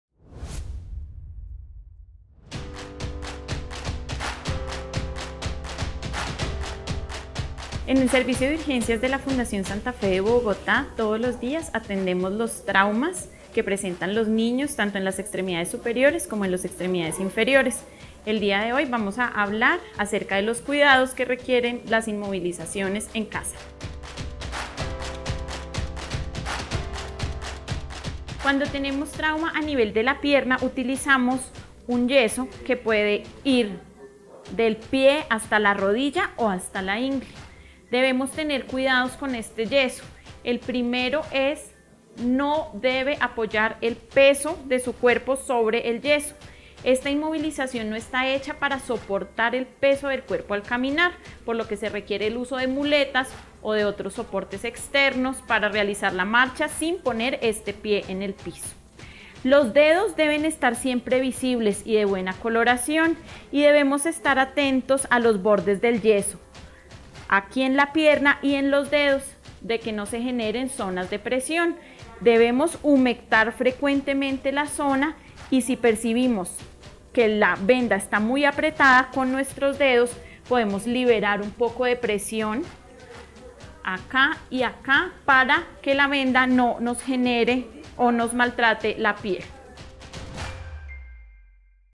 Tips de cuidado en casa: yeso en miembro inferior
Tips de Cuidado en Casa: comúnmente los más pequeños sufren accidentes, guarda este video para saber como cuidar un yeso en la pierna.

Para prevenir accidentes recuerda tener una alimentación adecuada y enseñar a los niños la prevención de golpes y caídas.